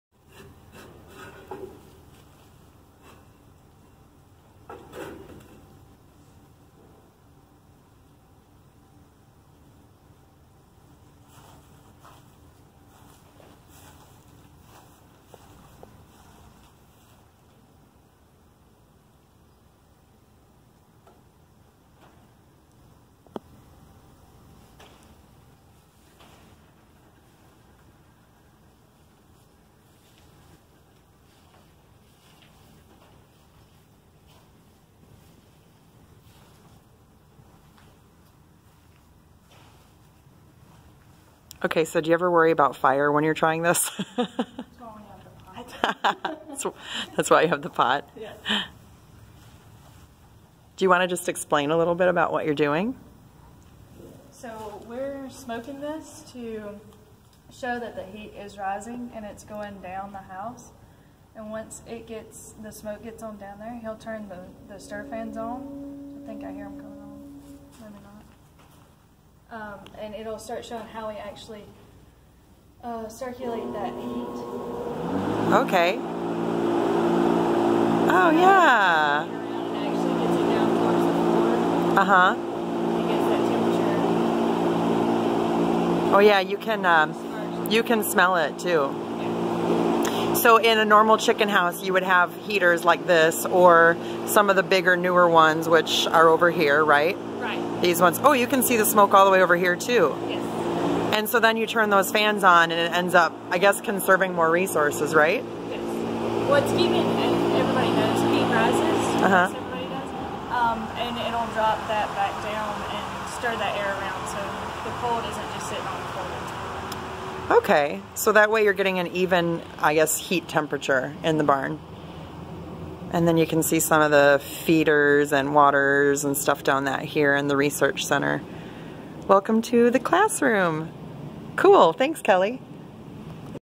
How large chicken barns control temperature!
A classroom simulation at Auburn University NPTC on how air is circulated to keep chickens comfortably warm